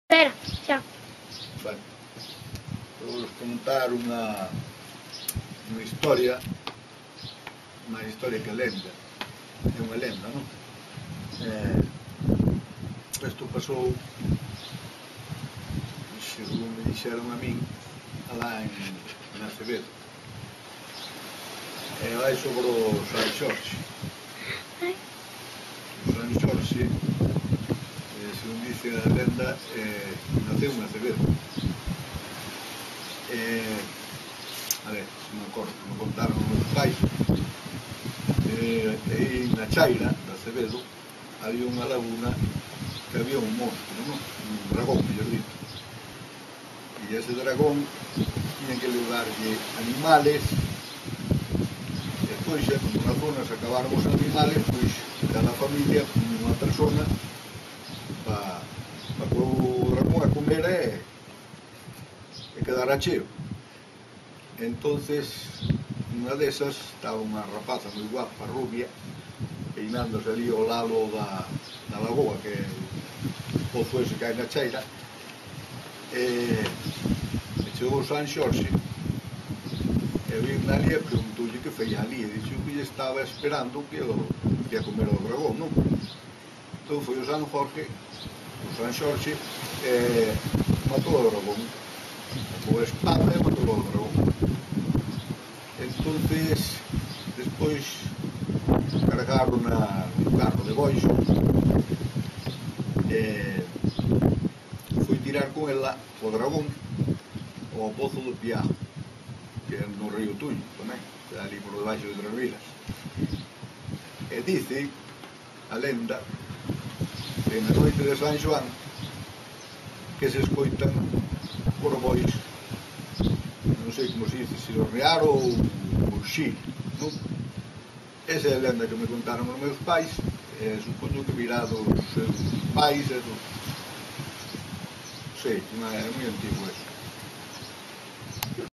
Colexio Sagrado Corazón (Celanova) - 2 - Como se vivía antes
Desde o Colexio Sagrado Corazón (Celanova) chéganos este material para o segundo certame de recolla da tradición oral, no curso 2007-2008, organizado por Ponte…nas Ondas!